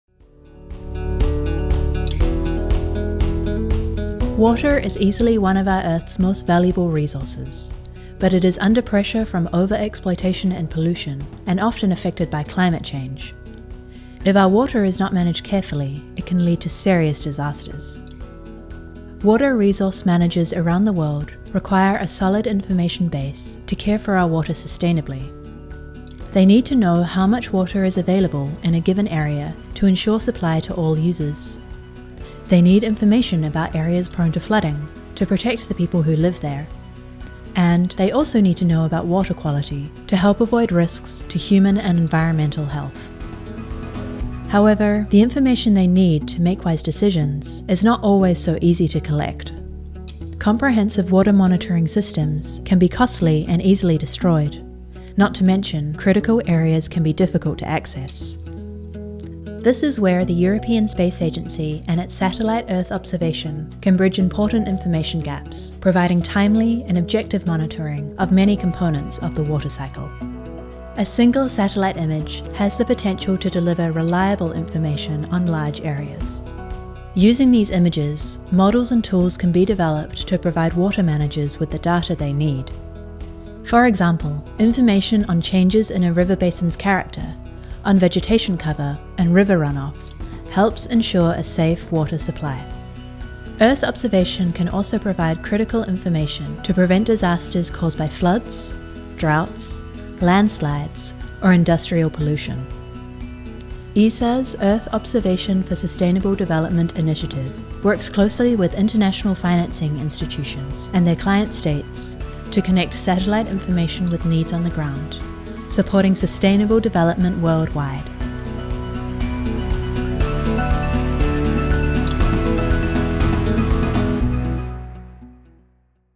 Helping to manage water
Collecting information on water resources at over large areas is often difficult. This limits national and river-basin water managers’ ability to make the appropriate decisions. Satellites can fill this information gap with timely observations. Since 2008, ESA has been working closely with international financing institutions and client states to harness the benefits of Earth observation for global sustainable development. Earth Observation for Sustainable Development (EO4SD) is a new ESA initiative that aims to increase the uptake of satellite information in regular development operations nationally and internationally.